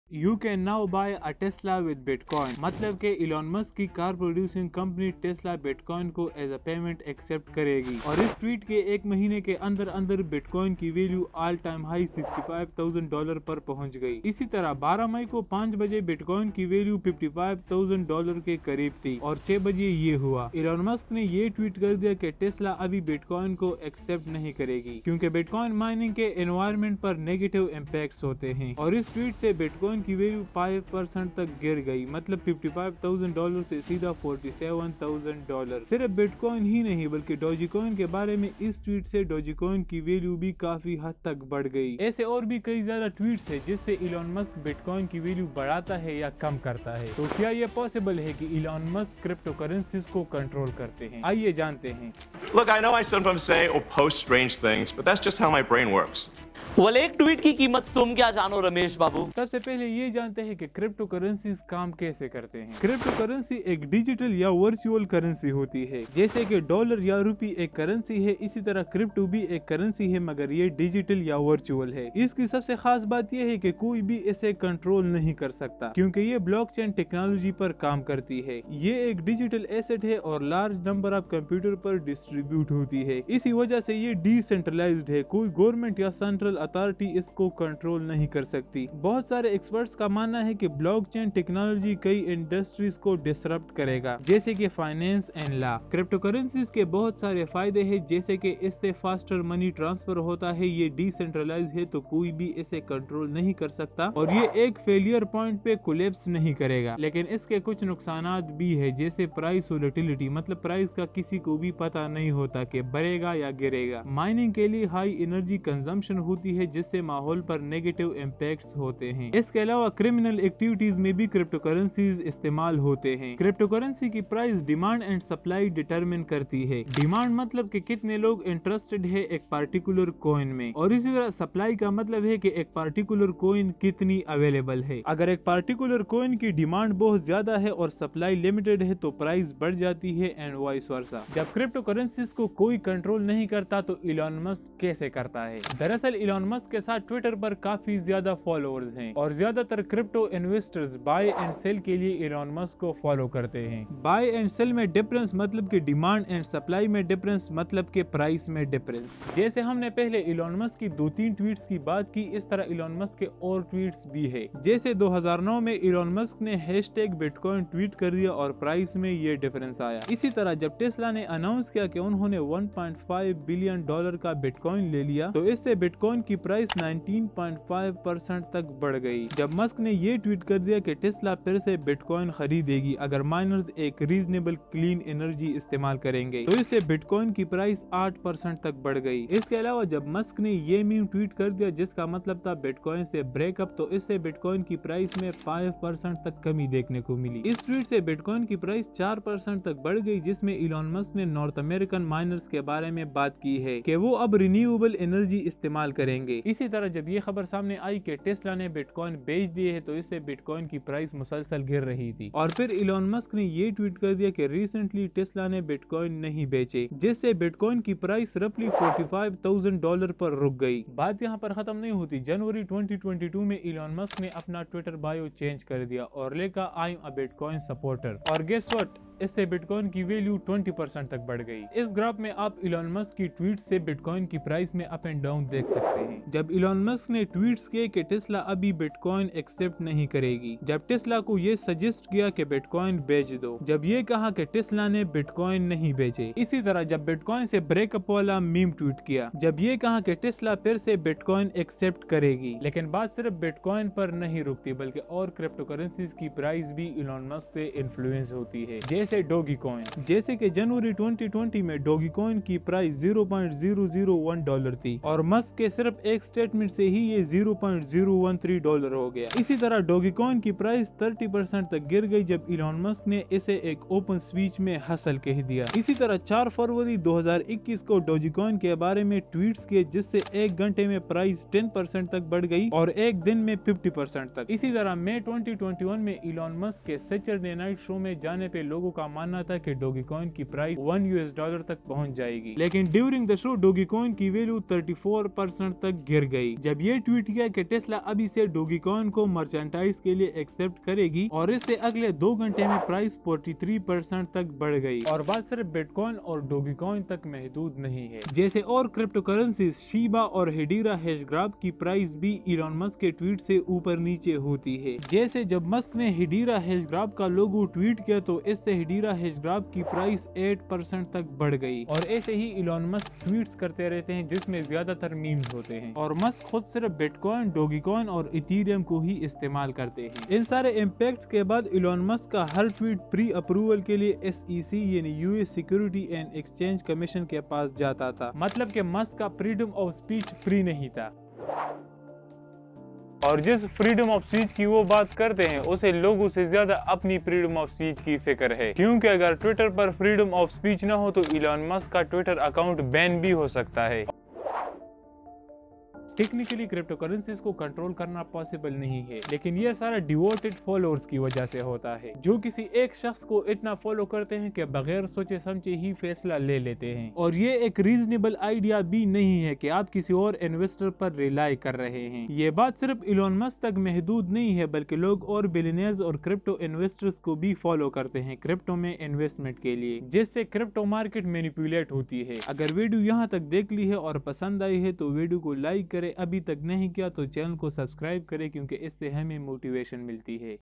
How Elon Musk Controls Cryptocurrencies | Elon Musk Manipulates Bitcoin & Dogecoin
About Video:
This video is all about how Elon Musk Controls Cryptocurrencies. Technically no one can control the crypto market but Elon Musk do it by his huge twitter following. More about this topic with brief and proofs in this video.

Timestamps⏳:
00:00 Intro
00:59 How Cryptocurrencies works
02:21 How Musk's tweets impacts Crypto
02:39 Impacts on Bitcoin
04:12 Impacts on Dogecoin
05:04 Impacts on other cryptocurrencies
05:30 Reason Behind Twitter takeover
05:54 Conclusion